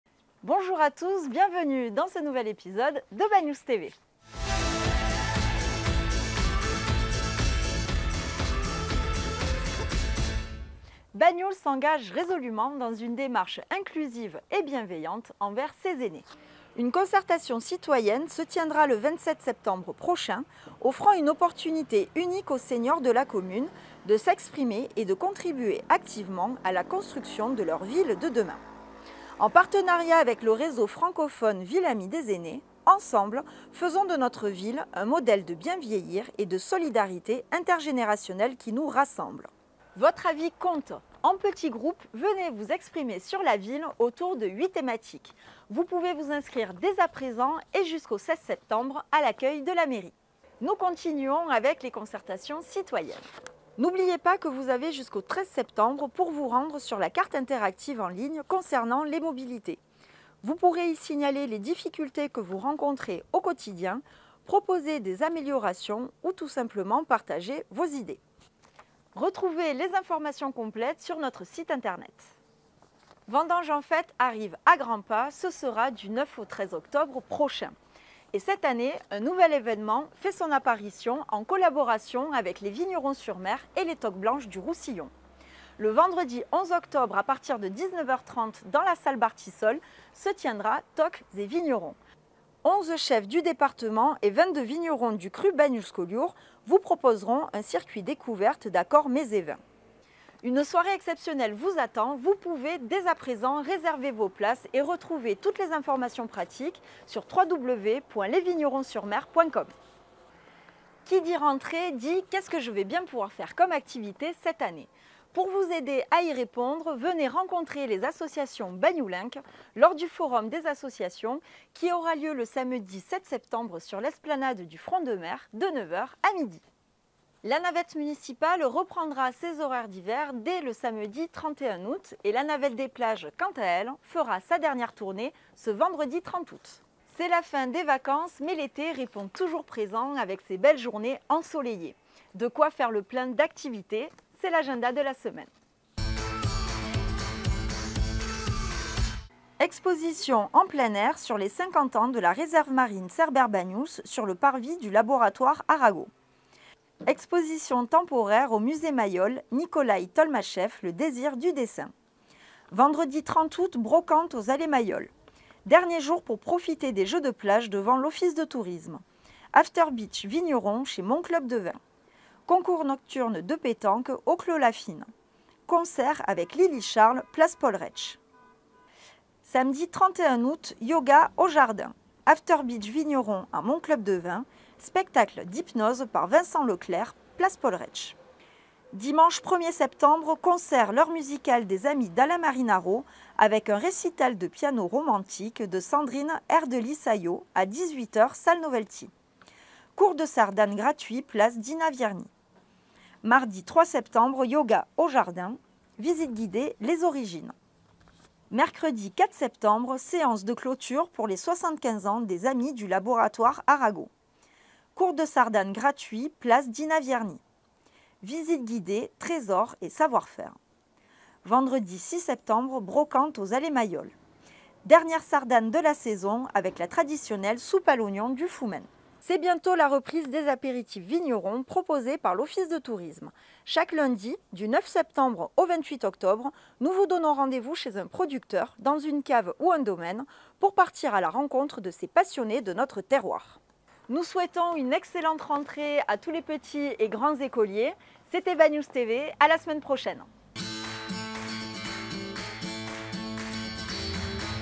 Banyuls Tv 3008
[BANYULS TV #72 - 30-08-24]

Nouvel épisode de Banyuls TV : des actualités et l'agenda de la semaine, c'est parti !

Cette semaine on parle concertations citoyennes avec Ville amie des aînés et Schéma des mobilités, nouvel événement Vendanges en Fête et Forum des associations.